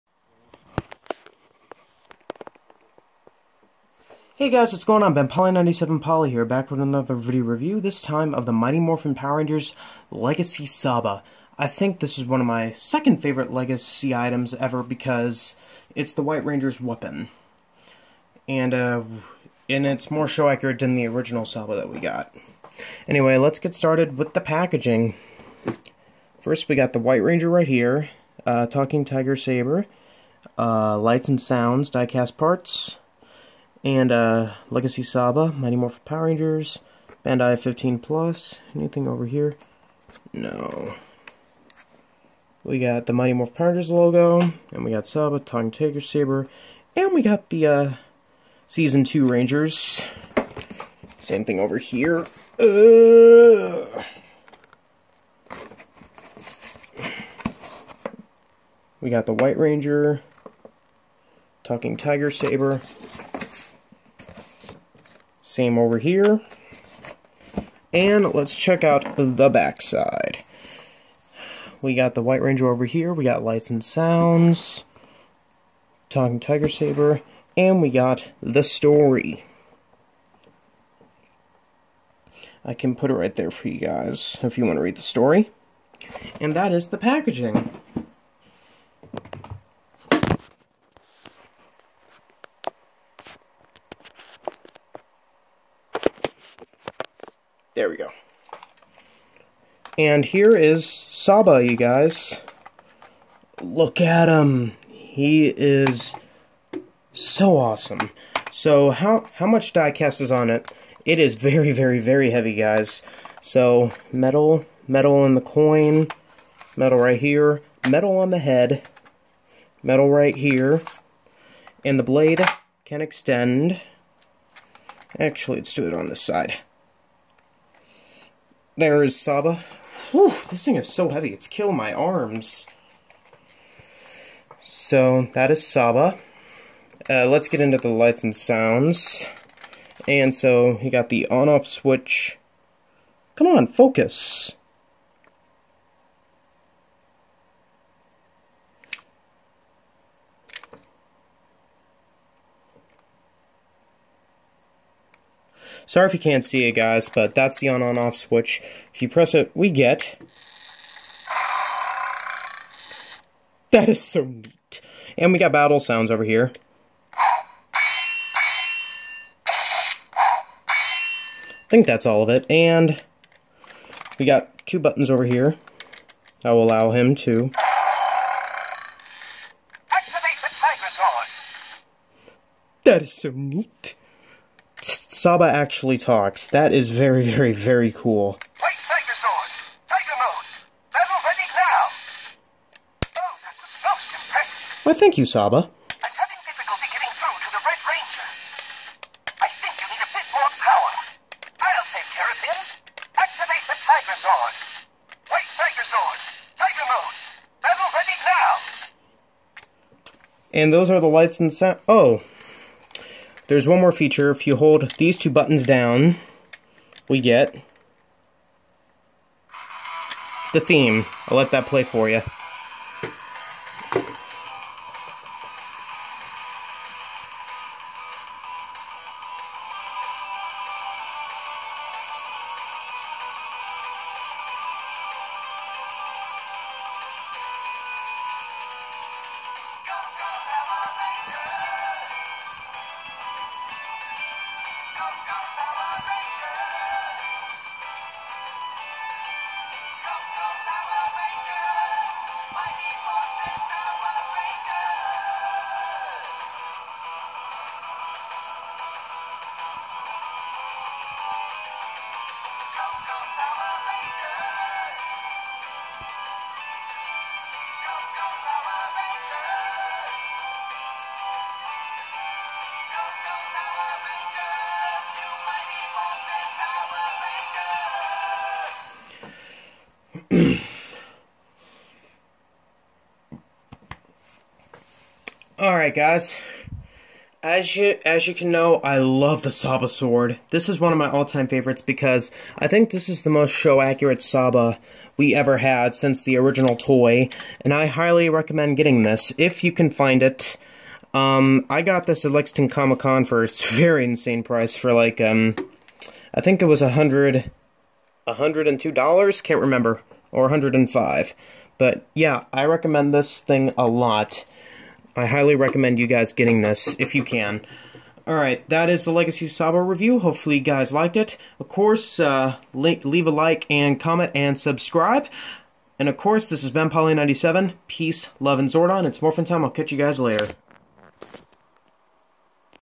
Toy Review: legacy saba
One of my all time favorite legacy items ever because its highly detailed and the die cast metal feels nice and the sounds are In incredible.